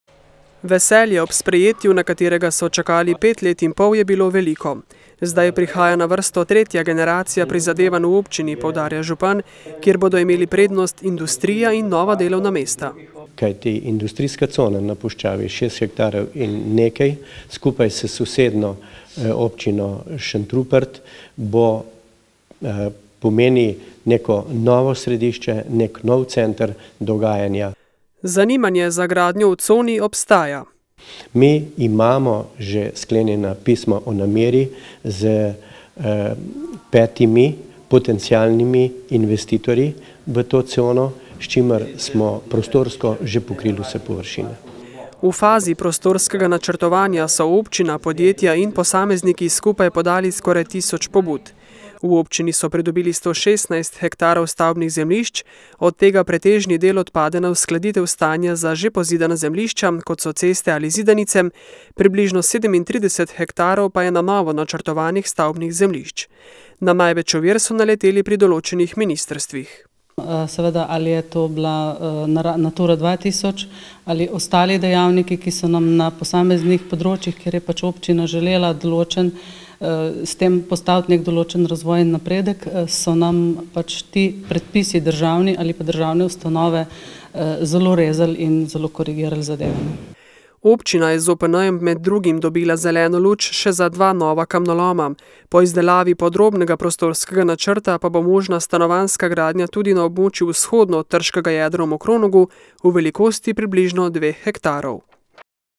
Z OPN-jem do razvojnih priložnosti
MOKRONOG - Eden najpomembnejših razvojnih dokumentov vsake občine je občinski prostorski načrt (OPN). Med tiste, ki ga imajo, zdaj uvrščamo tudi Občino Mokronog-Trebelno. Po napornih pripravah in usklajevanjih z nosilci urejanja okolja so ga namreč tamkajšnji občinski svetniki v drugi obravnavi soglasno sprejeli.